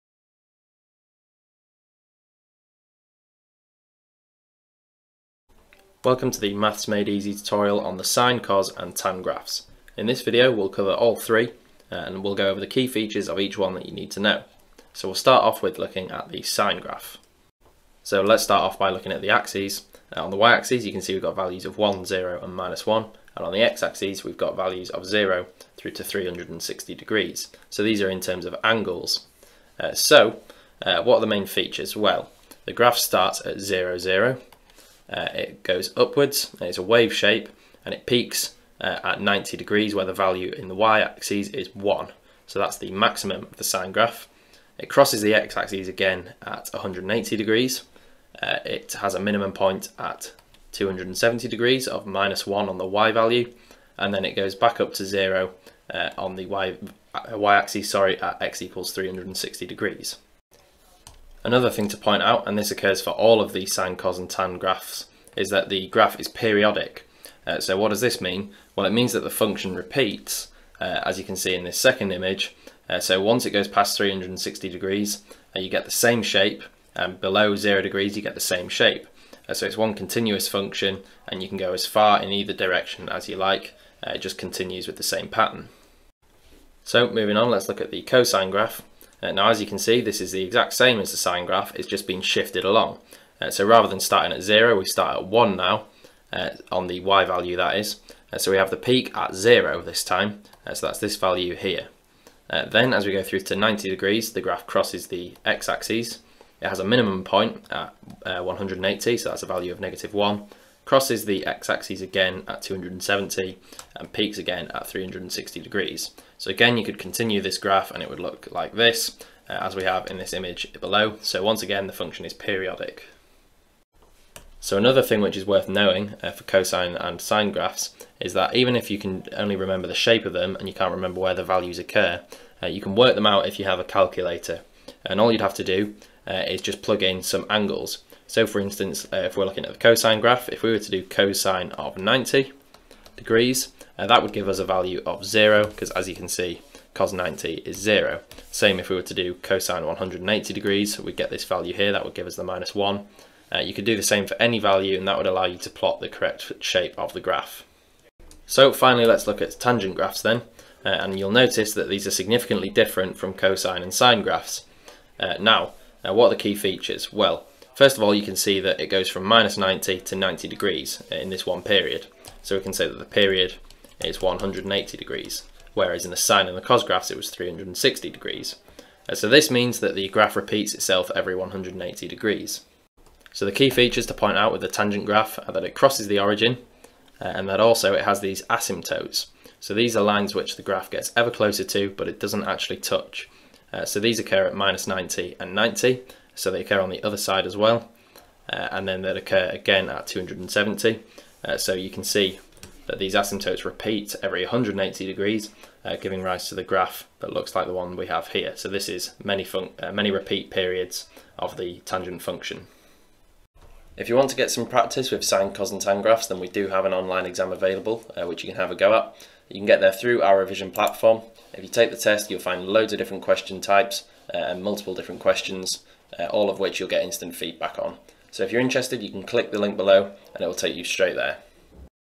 Graphs - Sin Cos and Tan Graphs
This is the Maths Made Easy video on Graphs - Sin Cos and Tan Graphs

To see our Graphs - Sin Cos and Tan Graphs online exam click the following

TIMESTAMPS
0:00 Intro
0:05 Sine Graphs
1:26 Cosine Graphs
2:38 Tangent Graphs
3:33 Outro